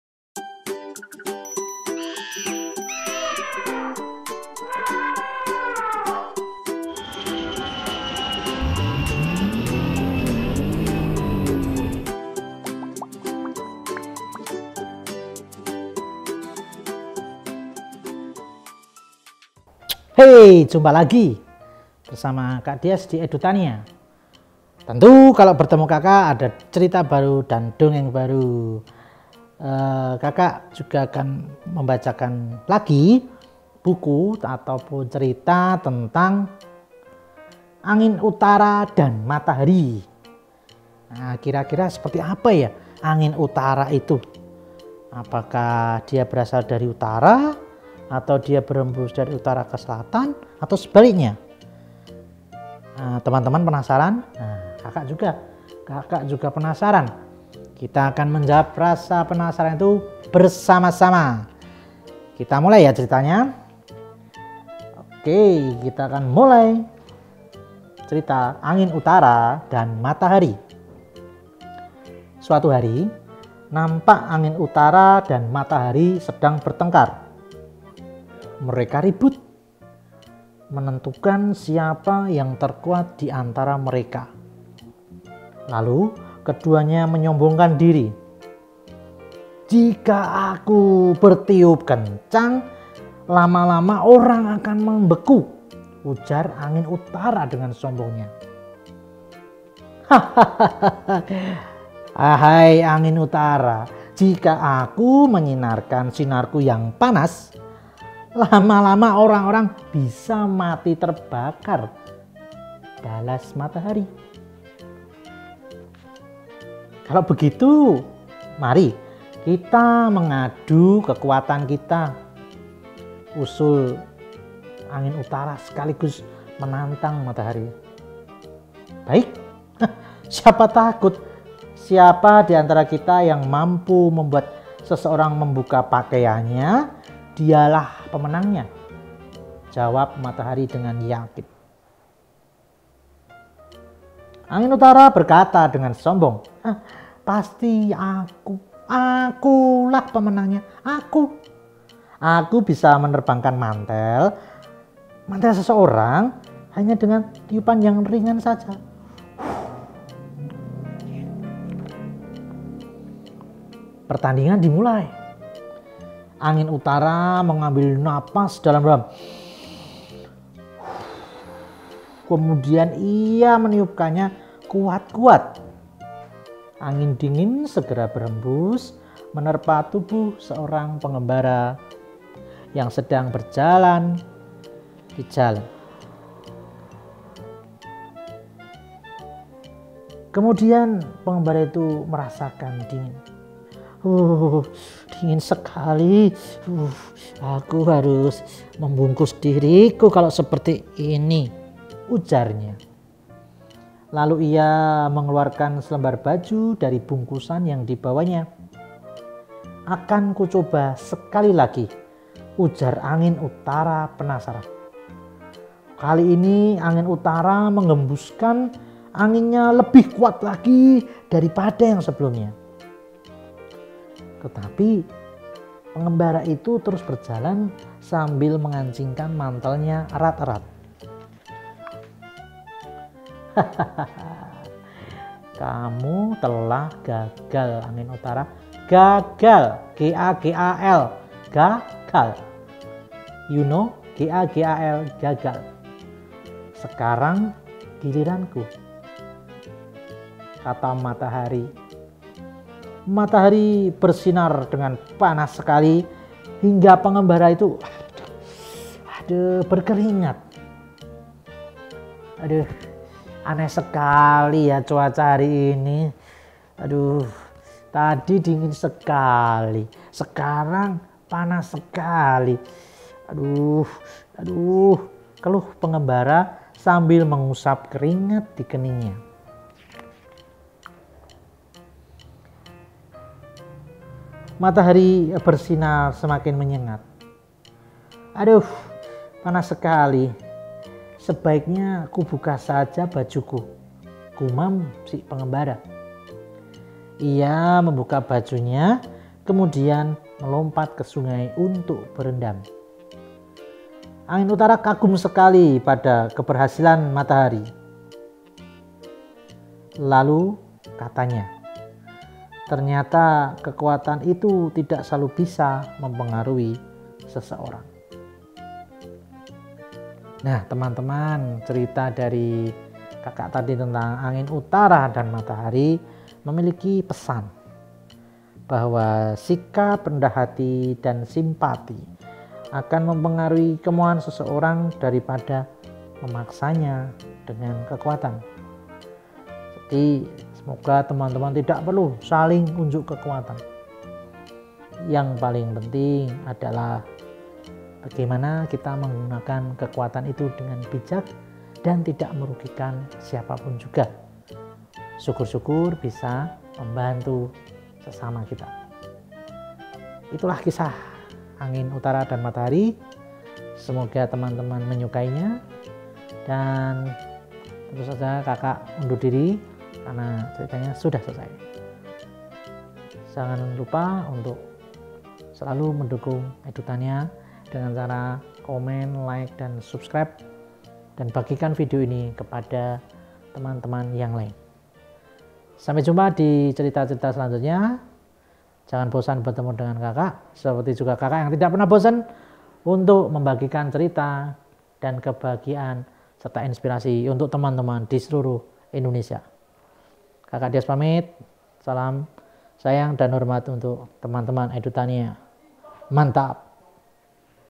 DONGENG MATAHARI DAN ANGIN UTARA | CERITA ANAK BERBAHASA INDONESIA
HAI...HAI...HAI...DONGENG MATAHARI DAN ANGIN UTARA PASA BANGET NIH DISIMAK KETIKA CUACA SEDANG TIDAK JELAS SEPERTI SEKARANG INI. YUK, DISIMAK DONGENG MATAHARI DAN ANGIN UTARA...